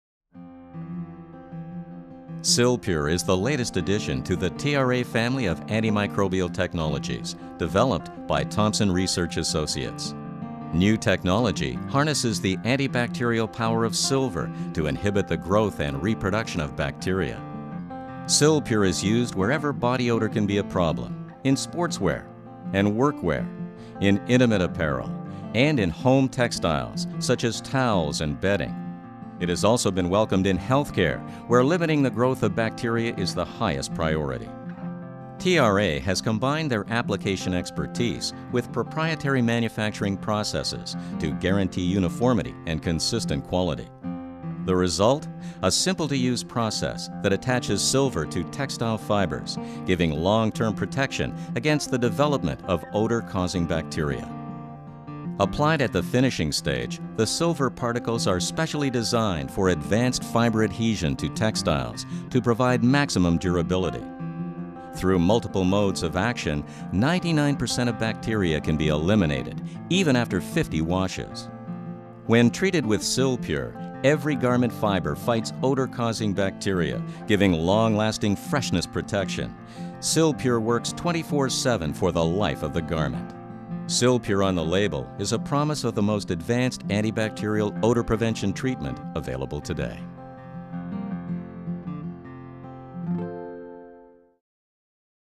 Silpure Antimicrobial Technology
Silpure is the latest addition to the TRA family of antimicrobial technologies, developed by Thomson Research Associates. New technology harnesses the antibacterial power of silver to inhibit the growth and reproduction of bacteria.